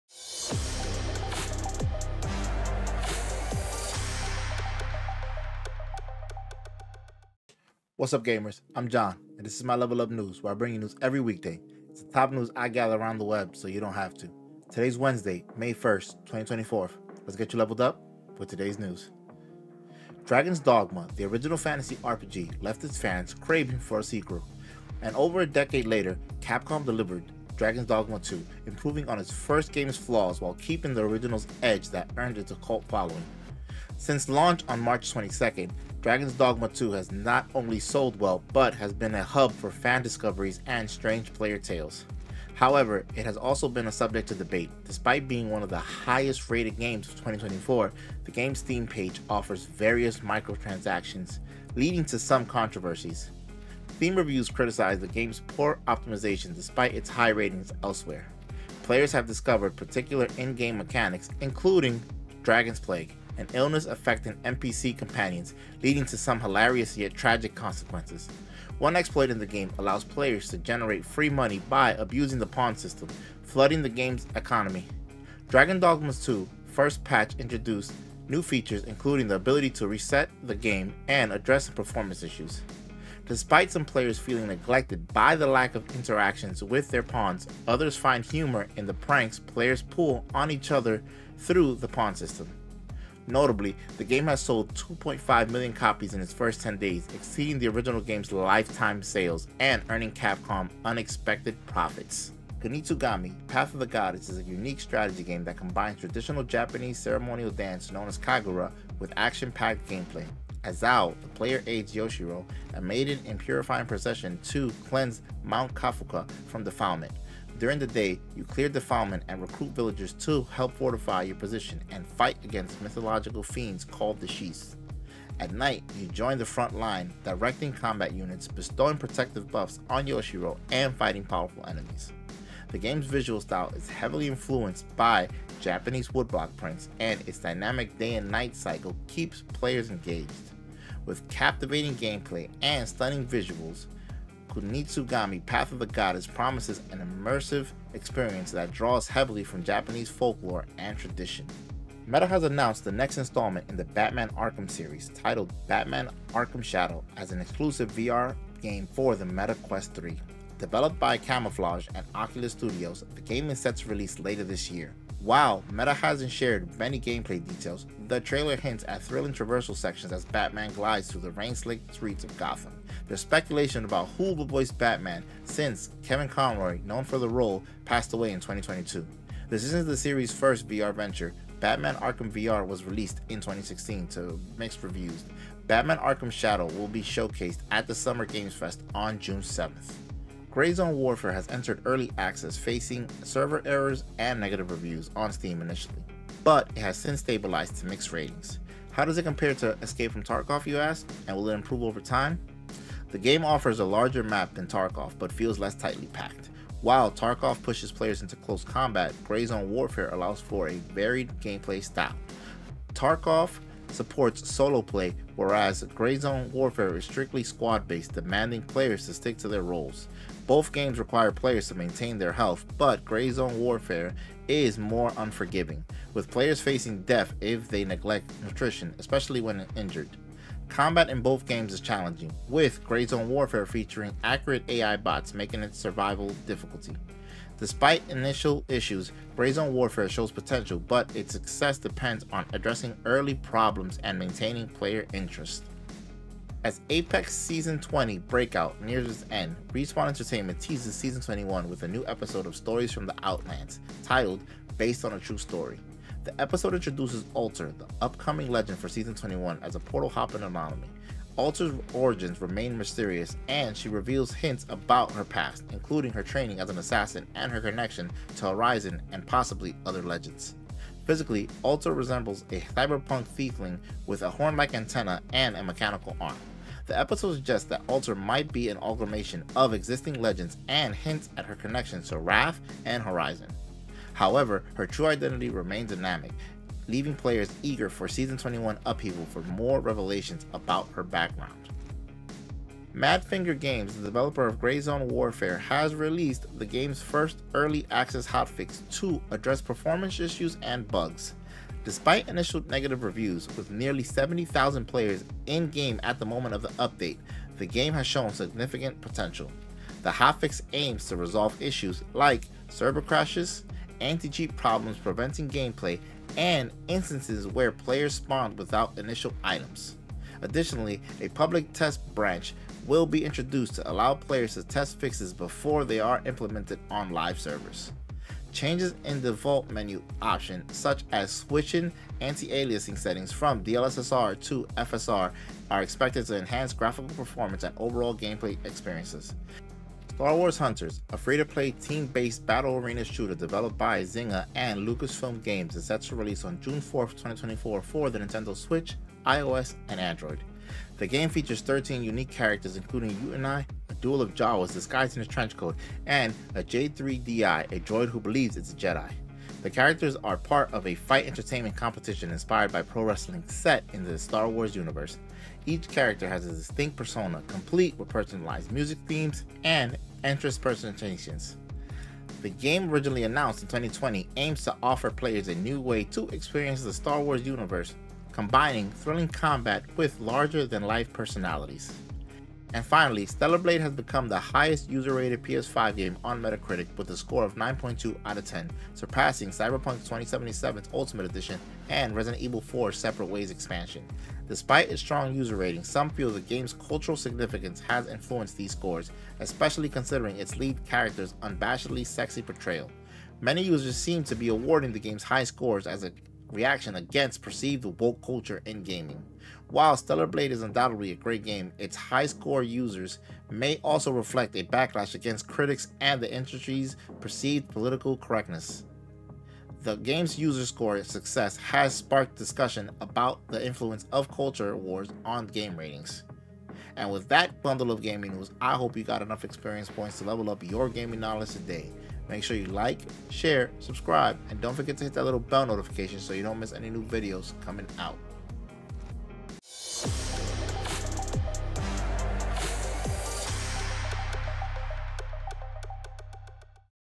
Stellar Blade scores high with players on Metacritic with a 9.2/10.
Stellar Blade Scores high with Players on Metacritic With a 9.2/10 Discover the latest updates in the gaming world! Dragon’s Dogma 2 has impressed fans with its improved gameplay and quirky tales since its release on March 22. Kunitsu-Gami: Path Of The Goddess offers a unique blend of traditional Japanese ceremonial dance and action-packed gameplay, promising an immersive experience inspired by Japanese folklore. Batman: Arkham Shadow, the next installment in the Batman: Arkham series, is an exclusive VR game for the Meta Quest 3, set to release later this year. Gray Zone Warfare, a strategic game set in a futuristic universe, has entered Early Access, addressing performance issues with its first hotfix. Apex Legends Season 21 introduces Alter, a mysterious new Legend, while Stellar Blade scores high player rated PS5 game on metacritic, scoring a 9.2/10, sparking discussions about cultural influences on game ratings. Finally, get ready for the release of Star Wars: Hunters, a free-to-play team-based battle arena shooter, promising an exciting new way to experience the Star Wars universe, coming to Nintendo Switch, iOS, and Android on June 4, 2024.
00:00 Introduction
00:20 Dragon's Dogma 2 Still fun
01:42 Kunitsu Gami Path of the Goddess
02:32 Meta X Batman
03:10 Grey Zone Vs Tarkov
04:06 Based on A True Story
04:52 Grey Zone Update
05:39 Star Wars Hunters
06:22 Stellar Blade a Metacritic Hit